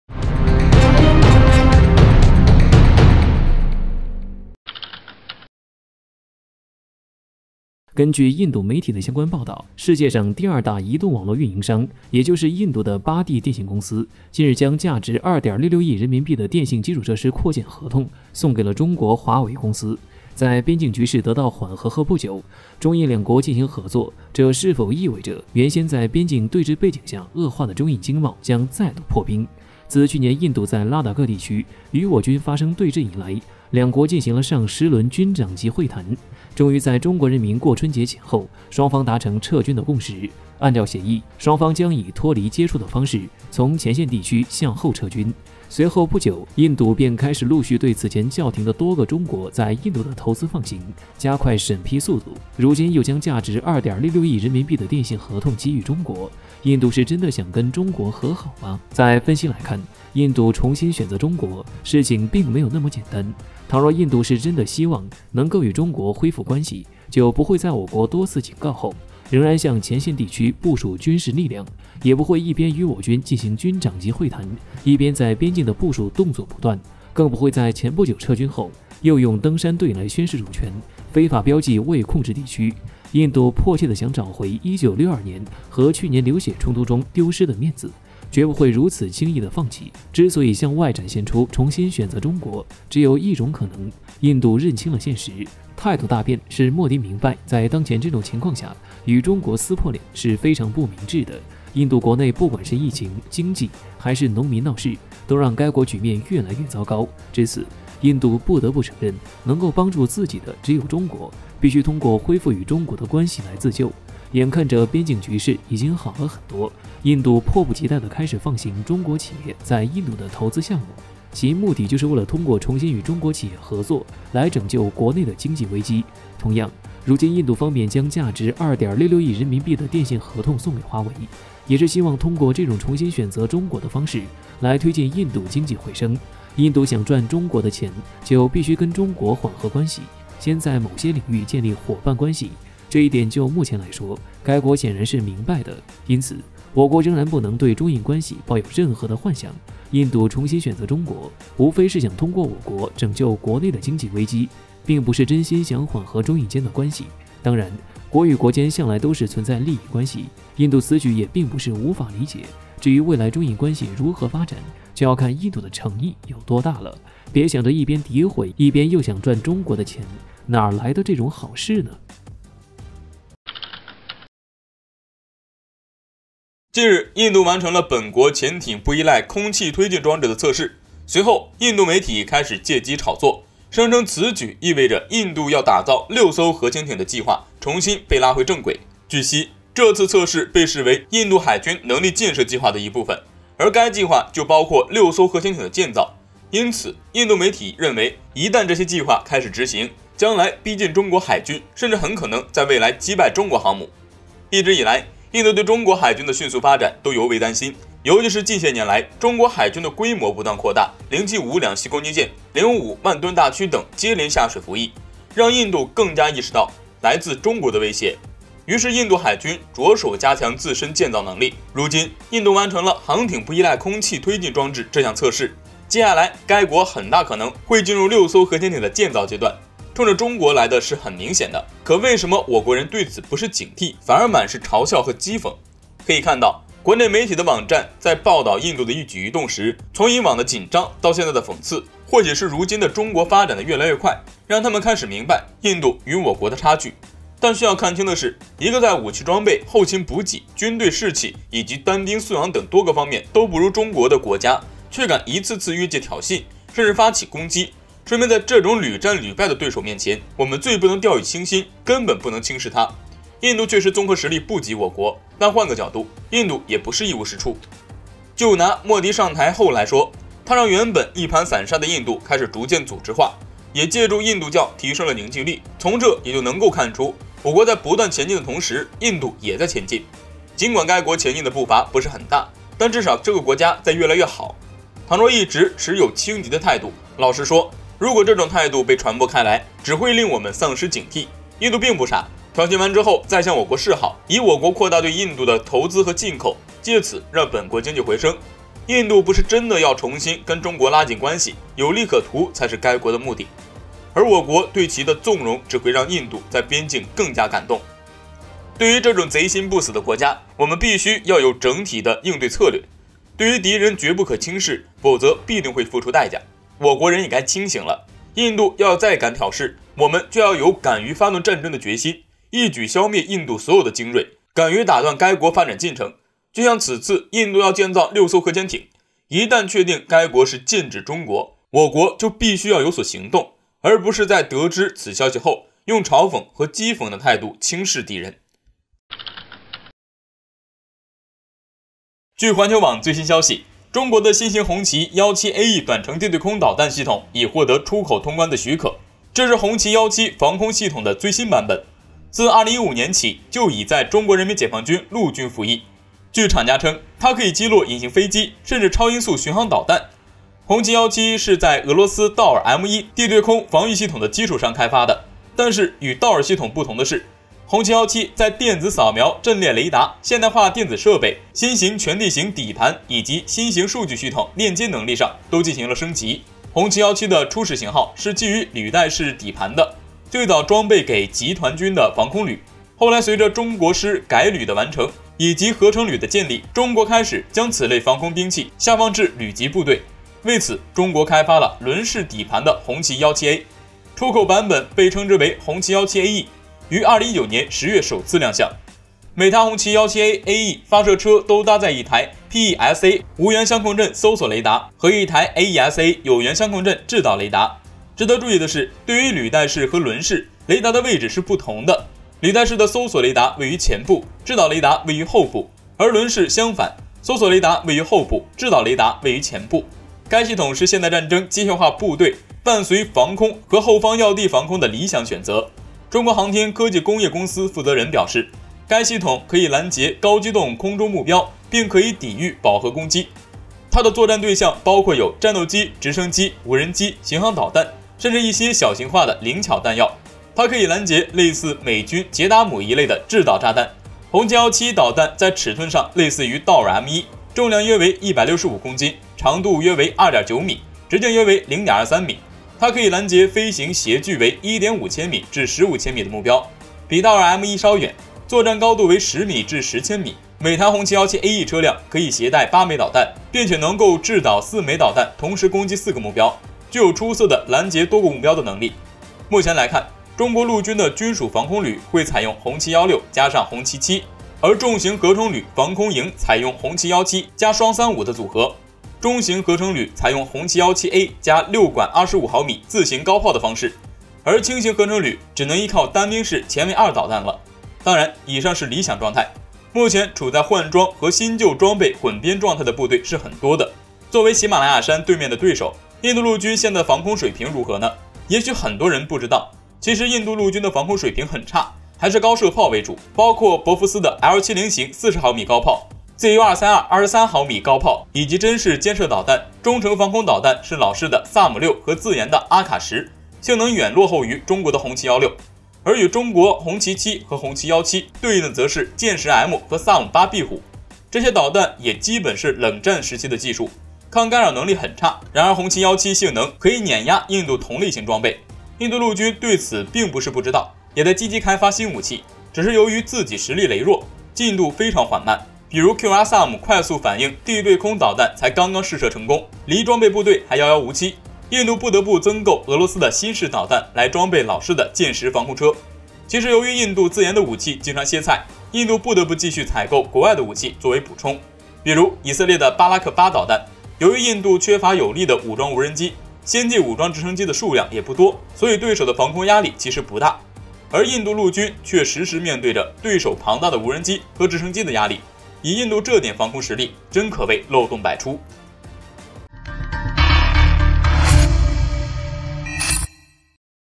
印度要打造6艘核潛艇，國人卻都是嘲笑和譏諷？喪失警惕會付出代價【强国军事】
1、00:00-02:53 將2.66億電信合同送給華為，印度重新選擇中國？事情沒那麼簡單
2、02:54-05:38 看貶印度會付出代價！要打造6艘核潛艇擊敗中國航母？看貶印度會付出代價！
3、05:39-09:34 紅旗-17AE短程地對空導彈系統獲出口許可，性能碾壓印度同類型裝備
印度要打造6艘核潛艇，國人卻都是嘲笑和譏諷？喪失警惕會付出代價
强国军事 军事 政治 我們的影片會遵循公平、尊重的原則，確保不會包含任何仇恨言論的元素。